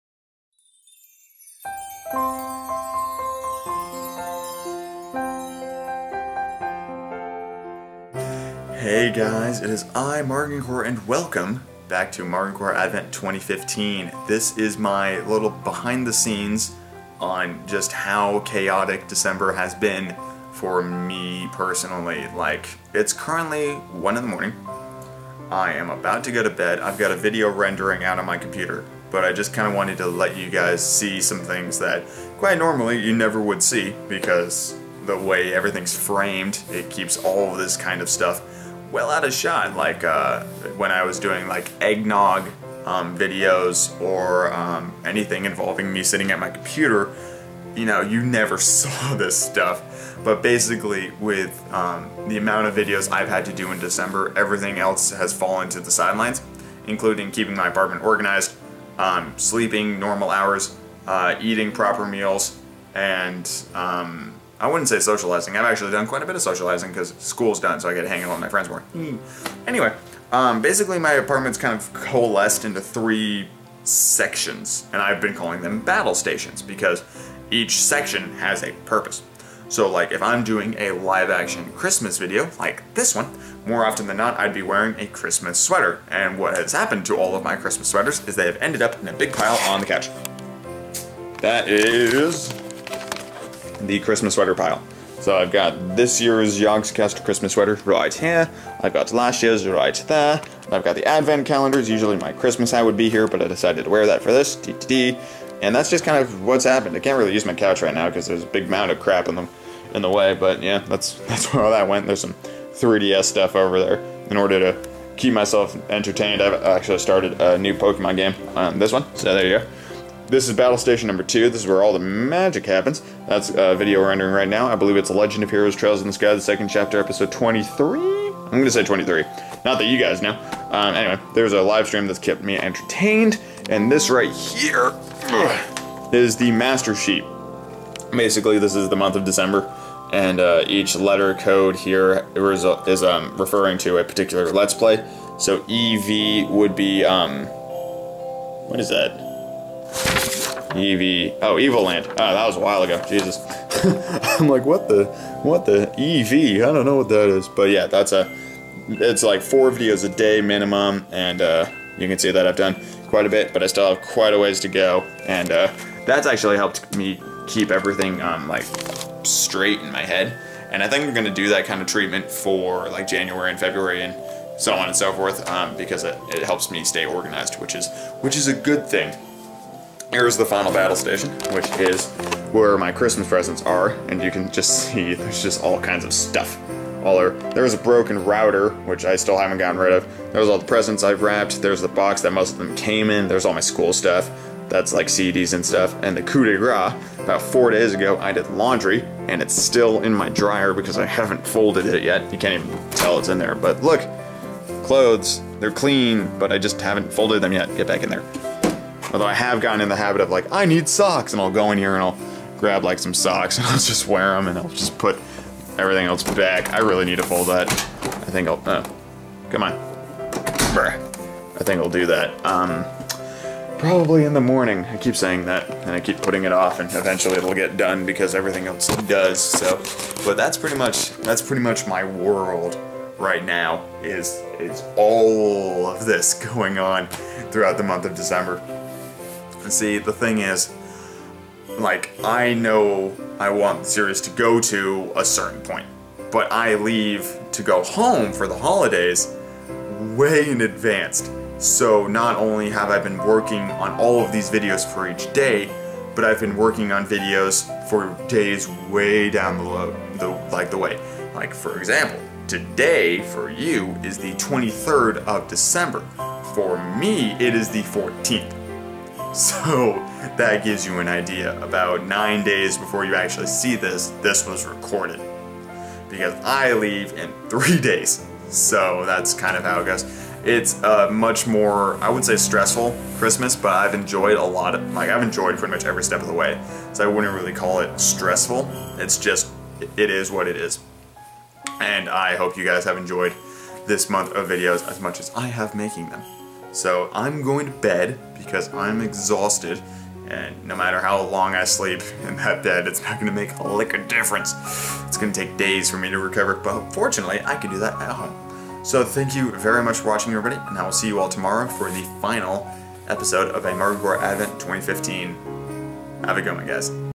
A Marginkor Advent 2015 - Day 23
Here's a quick behind the scenes look at what December has been like for me! Enjoy! :D

Theme Song From Monstercat Records.